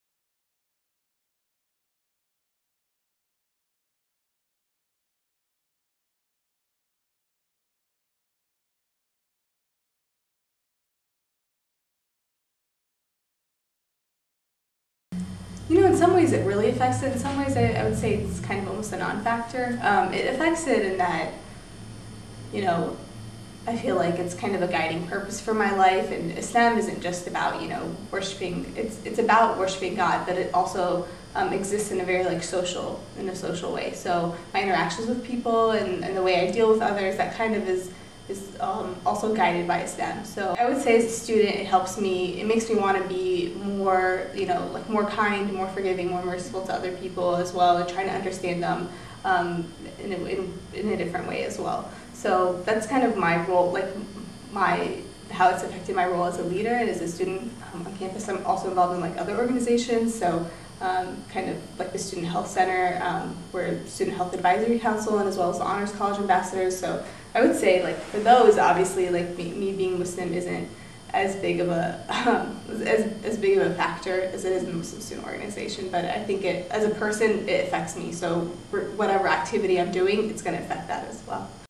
Stories of Faith - Life as a Muslim Collegiate Student
A University of Missouri student, Arwa explains what it's like being Muslim as a college student.

Part of a series of stories about faith and belief on the University of Missouri campus.